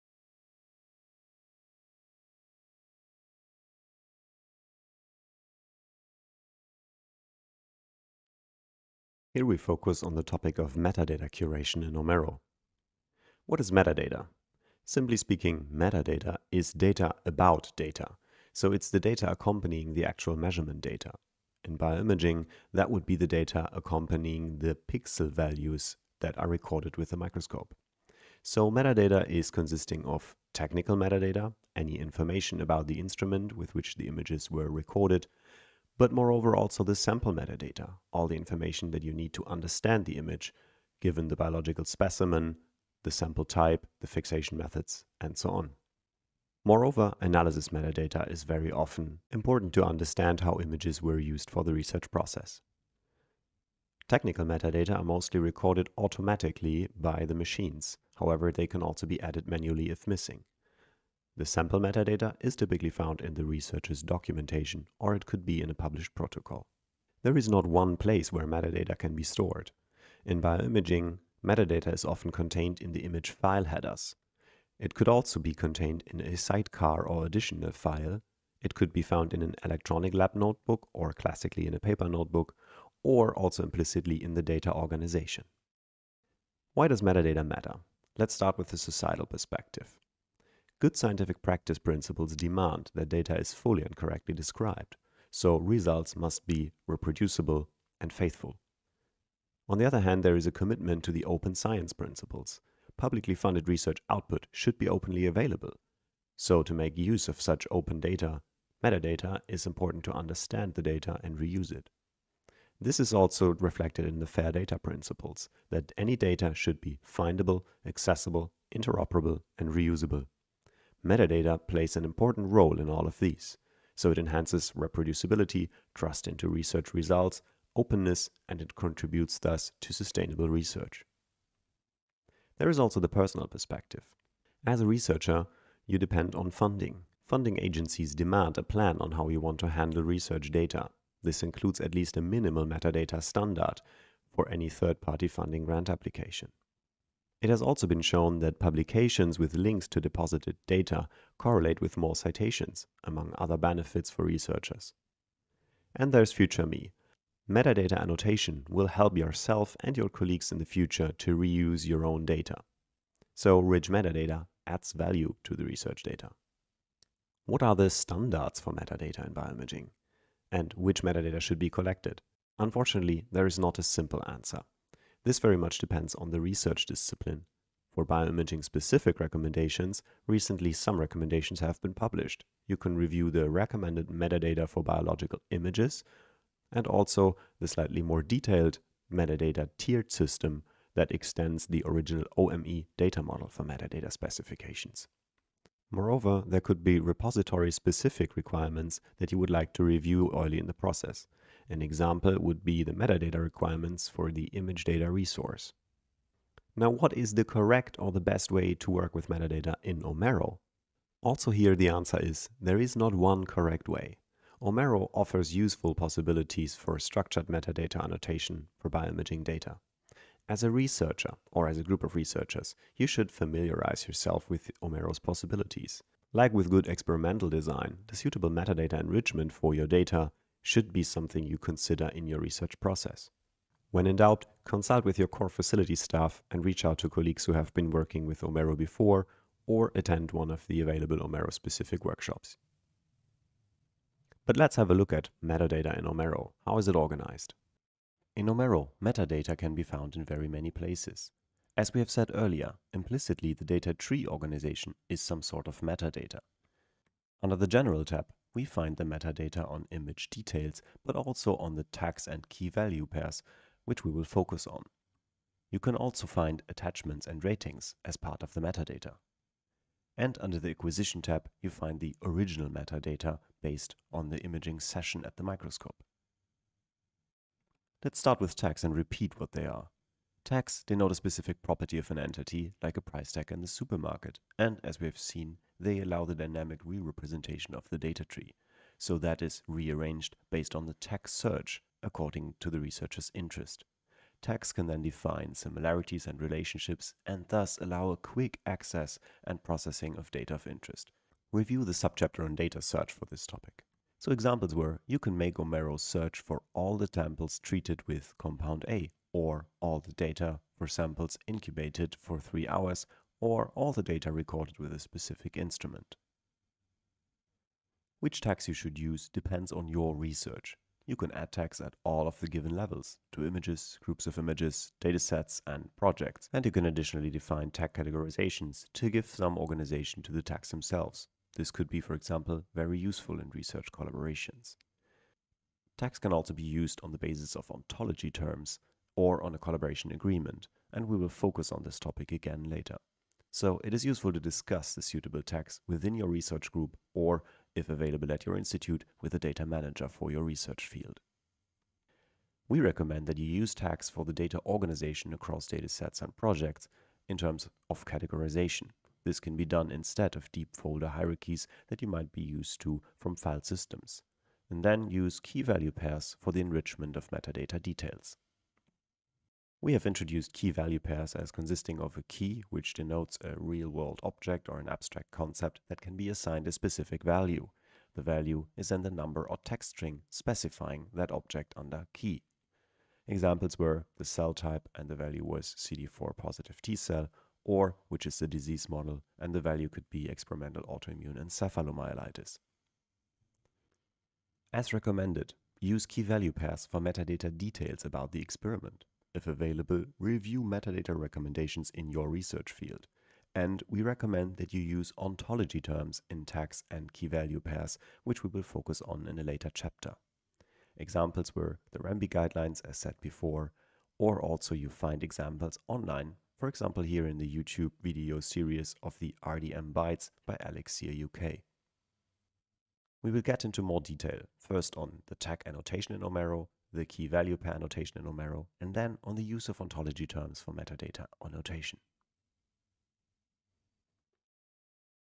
I3D:bio's OMERO Training Videos: 07.0 - Metadata (in OMERO)
Chapter 07.0 of the Playlist "I3D:bio's OMERO Training Videos 2023: Research Data Management for Microscopy".
Note that this chapter has three additional subchapters:
- 07.1 Tags in OMERO
- 07.2 Key-Value-Pairs in OMERO
- 07.3 Ontologies (in OMERO)

The underlying slides are openly available for reuse on Zenodo under the DOI: 10.5281/zenodo.8323588.

The project Information Infrastructure for BioImage Data (I3D:bio) was funded by the Deutsche Forschungsgemeinschaft (DFG, German Research Foundation).

for official documentation about OMERO.